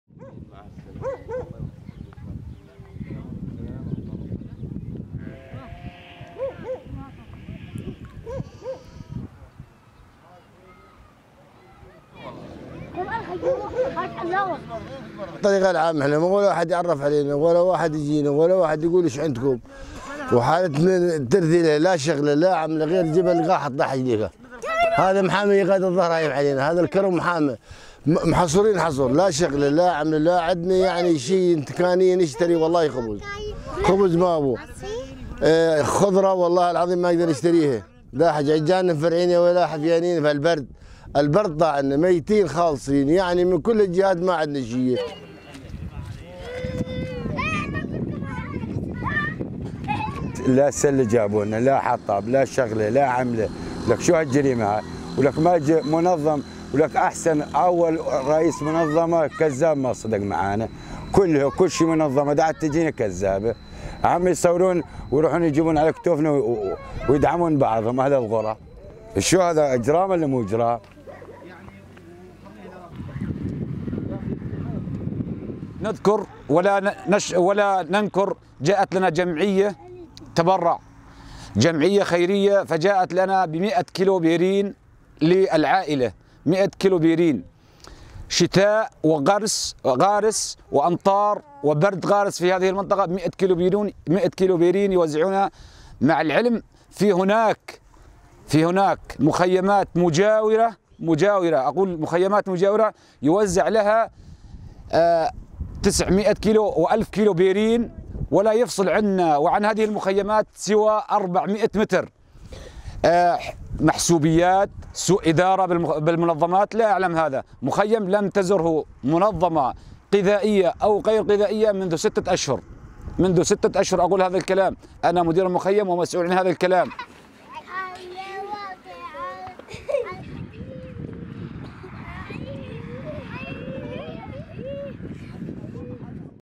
نازحون يعانون أضاع إنسانية صعبة في مخيم جنوب إدلب
يشتكي نازحون في مخيم "الفداء" جنوبي محافظة إدلب شمالي سوريا ظروفا إنسانية صعبة في فصل الشتاء. وقال مدير المخيم رياض الصايل الجمعة 8 شباط 2019، إن جمعية خيرية واحدة فقط زرات المخيم وجلبت لهم مئة كيلو بينزين للعائلات جميعها في هذا البرد القارس.وشهدت المخيمات في الآونة الأخيرة احتراق عدد من الخيام وإصابة قاطنيها نتيجة استخدامهم مواد ووسائل بدائية غير صالحة للتدفئة. الشخصيات حسب الظهور: 1 – النازح مصلح المحمد، 2 – النازح "أبو يوسف"، 3 – مدير المخيم رياض الصايل